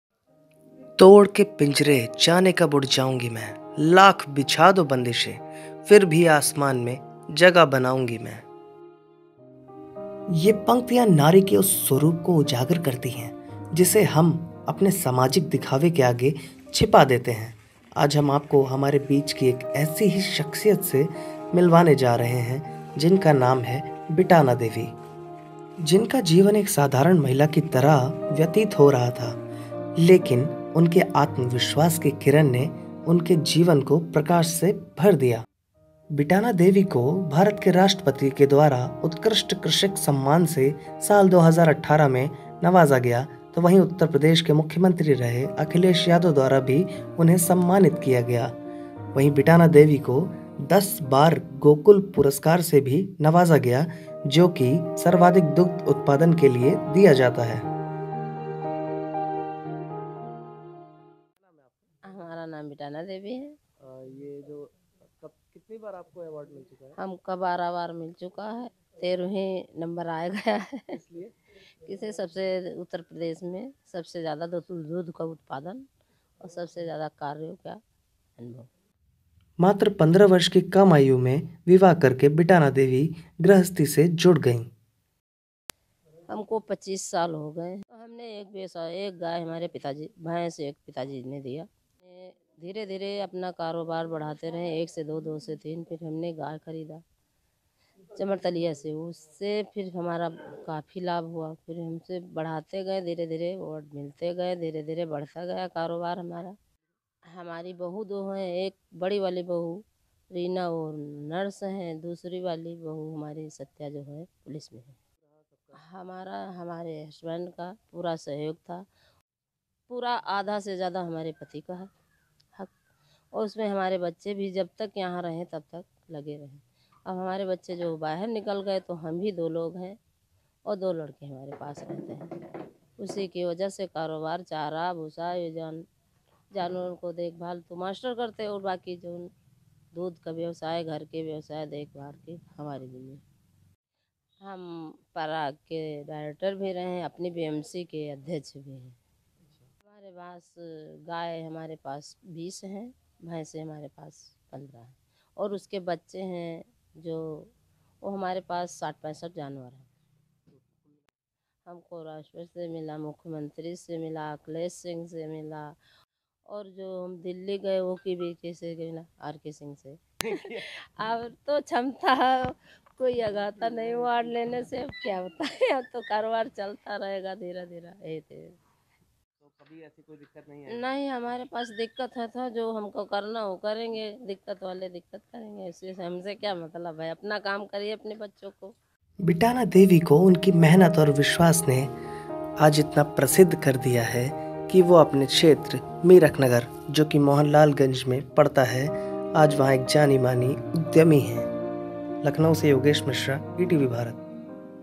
मिसाल : 10 बार गोकुल पुरस्कार से नवाजी जा चुकी हैं महिला उद्योगी बिटाना देवी
लखनऊ : 'तोड़ के पिंजरा जाने, कब उड़ जाऊंगी मैं, लाख बिछा दो बंदिशें, फिर भी आसमान में जगह बनाऊंगी मैं'... जी हां, ये पंक्तियां नारी के उस स्वरूप को उजागर करती हैं, जिसे हम अपने सामाजिक दिखावे के आगे छुपा देते हैं.
आज हम आपको हमारे बीच की एक ऐसी ही शख्सियत के बारे में बताने जा रहे हैं, जिनका नाम बिटाना देवी है. उन्हें सर्वाधिक दुग्ध उत्पादन के लिए राष्ट्रपति, मुख्यमंत्री द्वारा सम्मानित किया गया. बिटाना देवी को 10 बार गोकुल पुरस्कार से भी सम्मानित किया गया.